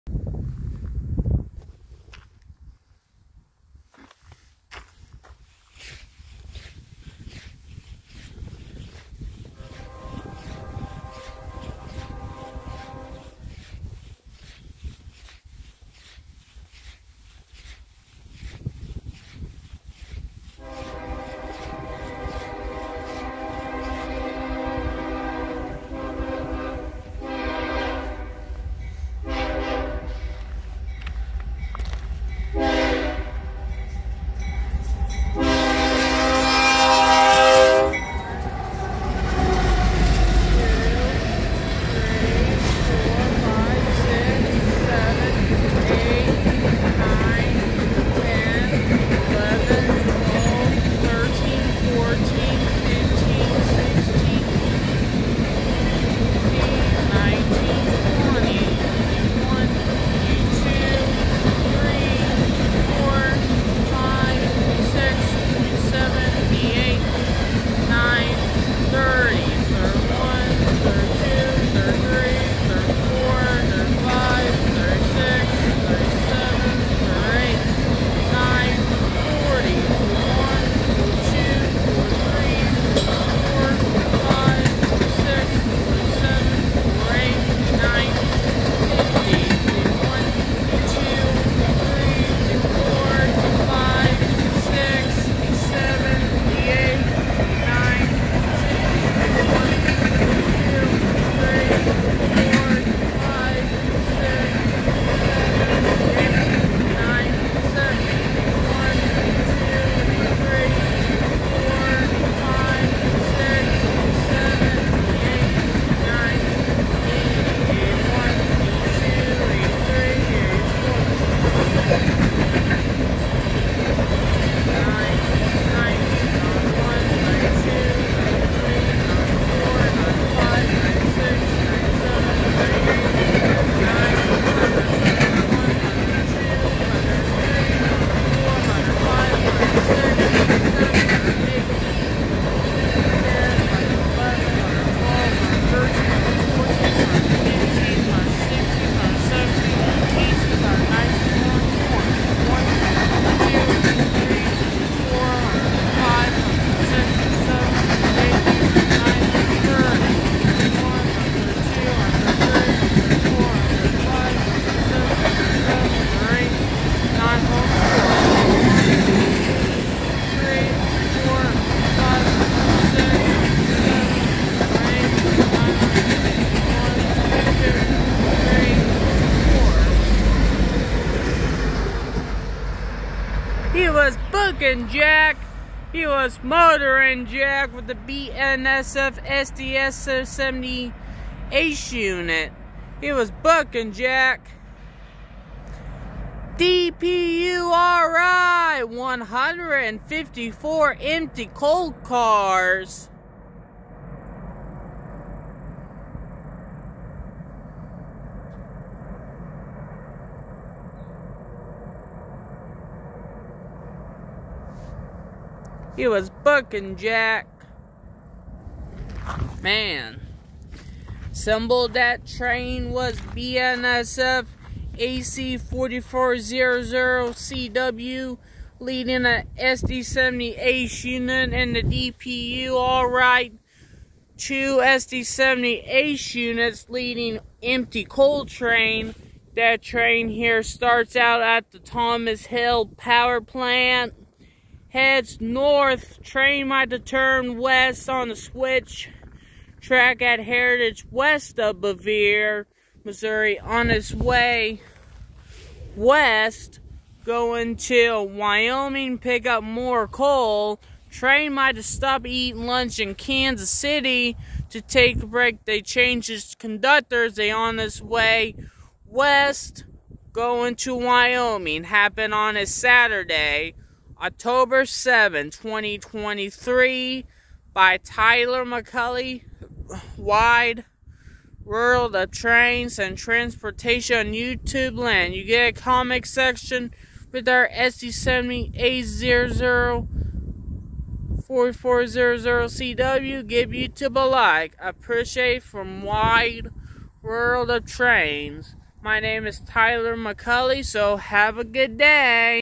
Railfanning Brookfield Sub BNSF 5628 9083 2 DPU alright lead Empty Coal Train near Callao MO 10/7/23
I was at Callao MO by Brookfield Subdivision I Saw Green Singal Westbound at Grade Singal Showing Green Westbound at Fireworks gravel road Here I Saw BNSF #5628 #9083 2 DPU alright leads Empty Coal Train pull 154 cars going Westbound past Fireworks gravel road in West Callao MO by Brookfield Subdivision On Saturday October 7th 2023 by Tyler McCully Wide world of Trains and Transportation YouTubeland.

BNSF #5628 #9083 lead Empty Coal Train Originated at Enegry Center at Thomas Hill MO at Power Plant heads West to Stop North Kansas City MO to North Antelope Rochelle Mine WY

BNSF : AC44CW  #5028
BNSF : SD70ACe #9083
DPU  alright BNSF SD70ACE  #8490
DPU alright  BNSF : SD70ACe  #9207

All Empty Coal Cars Unload Coal at Enegry Center Thomas  Hill MO to stop at North Kansas City MO to North Antelope Rochelle Mine WY

Making electricity